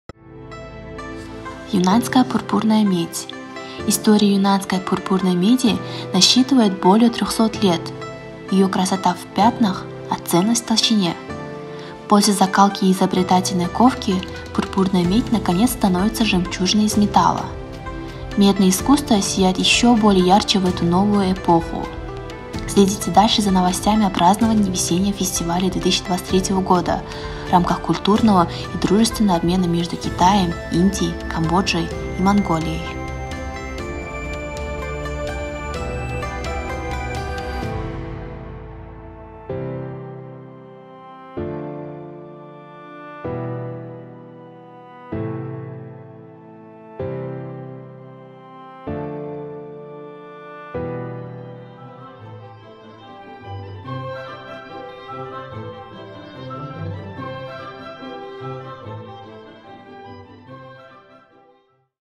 Юньнаньская пурпурная медь
История юньнаньской пурпурной меди насчитывает более 300 лет, ее «красота - в пятнах, а ценность - в толщине». После закалки и изобретательной ковки пурпурная медь, наконец, становится «жемчужиной из металла». Медное искусство сияет еще более ярче в эту новую эпоху.
Следите дальше за новостями о "Праздновании весеннего фестиваля 2023 года" в рамках культурного и дружественного обмена между Китаем, Индией, Камбоджей и Монголией.